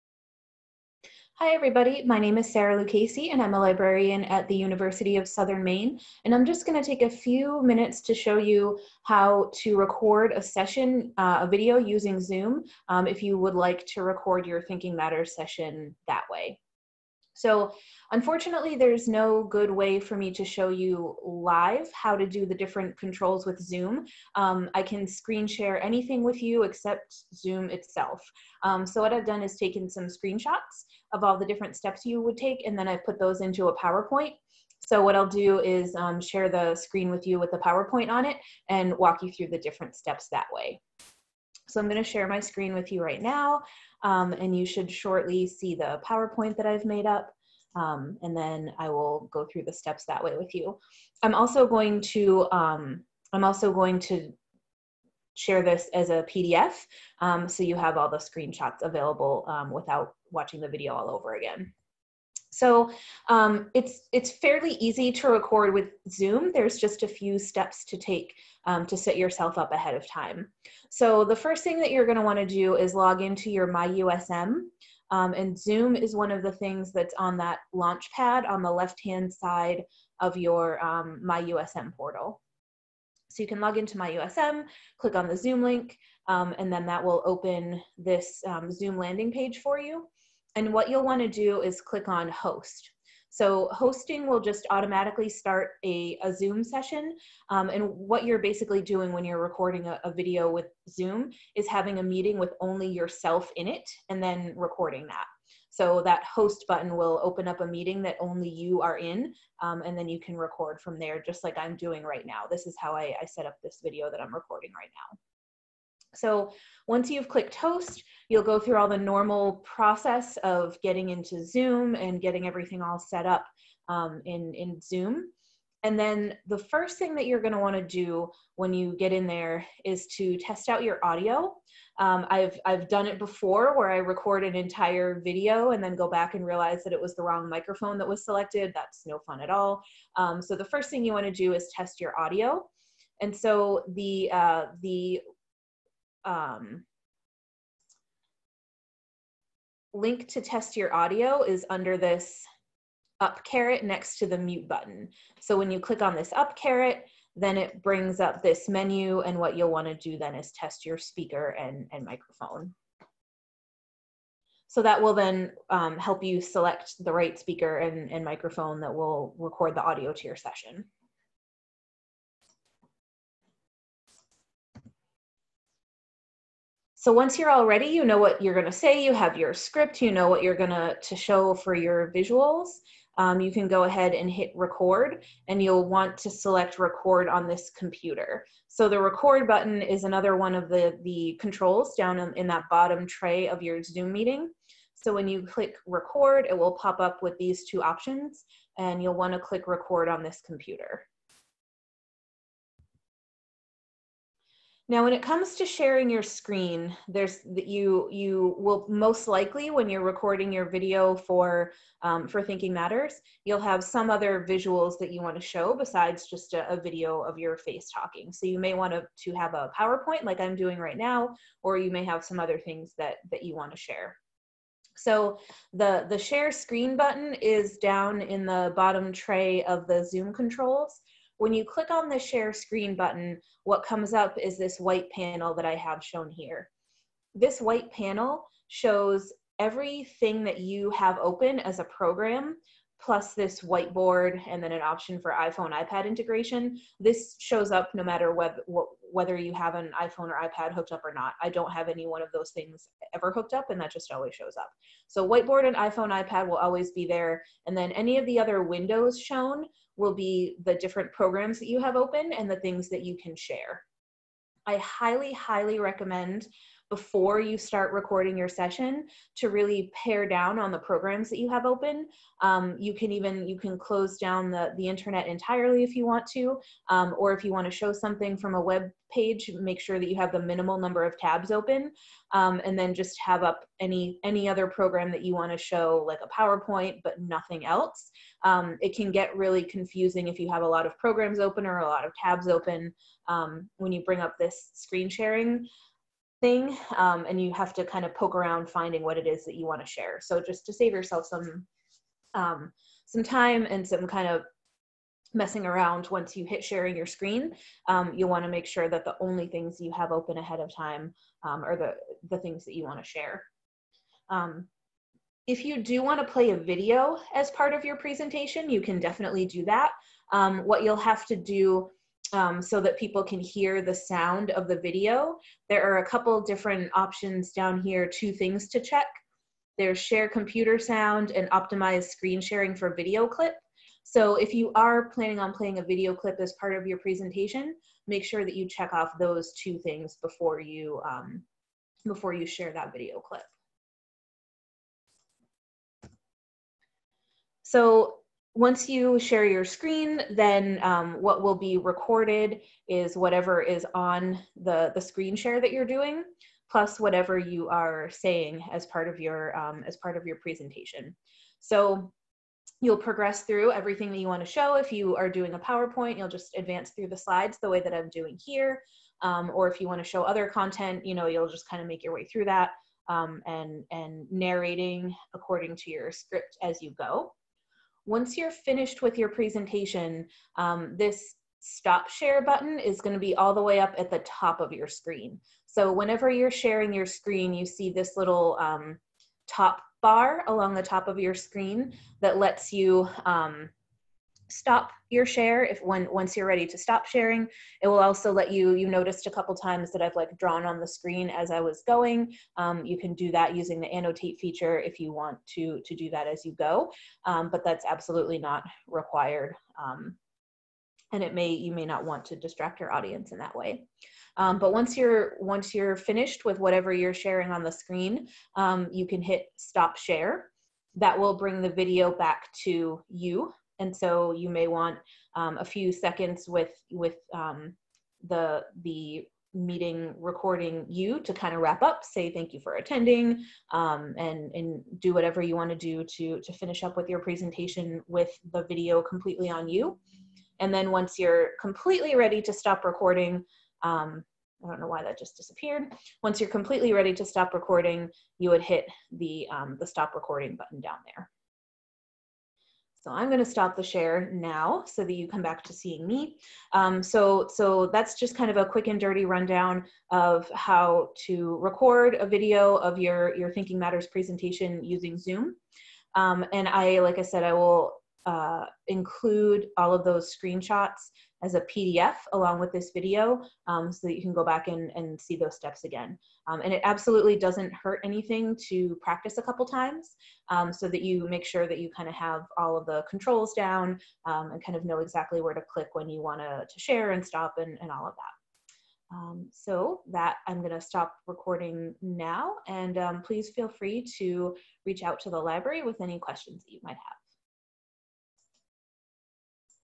Recording with Zoom
A basic introduction for recording with Zoom with Sarah Lucchesi, Coordinator of Research and Instruction Services at USM Libraries.